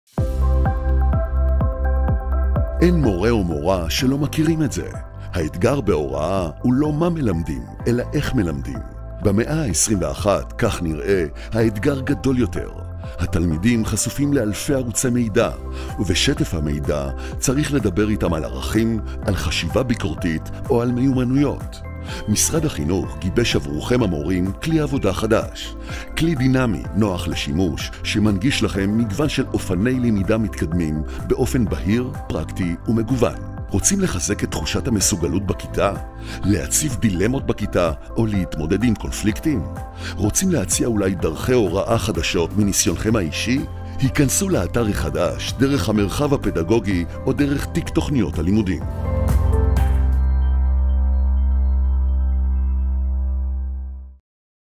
פרקטיקות הוראה מתקדמות – משנים את דרכי הלמידה
אין מורה או מורה שלא מכירים את זה:
האתגר בהוראה הוא לא מה מלמדים, אלא איך מלמדים.

במאה ה-21, כך נראה, האתגר גדול יותר: התלמידים חשופים לאלפי ערוצי מידע,
ובשטף המידע, צריך לדבר איתם על ערכים, על חשיבה ביקורתית, או על מיומנויות.

משרד החינוך גיבש עבור עובדי ההוראה כלי עבודה חדש. כלי דינמי, נוח לשימוש, שמנגיש מגוון של אופני למידה מתקדמים, באופן בהיר, פרקטי ומגוון.

רוצים לחזק את תחושת המסוגלות בכיתה?
להציף דילמות בכיתה, או להתמודד עם קונפליקטים?

רוצים להציע אולי דרכי הוראה חדשות, מניסיונכם האישי?

היכנסו לאתר החדש,
דרך המרחב הפדגוגי,
או דרך תיק תכניות הלימודים.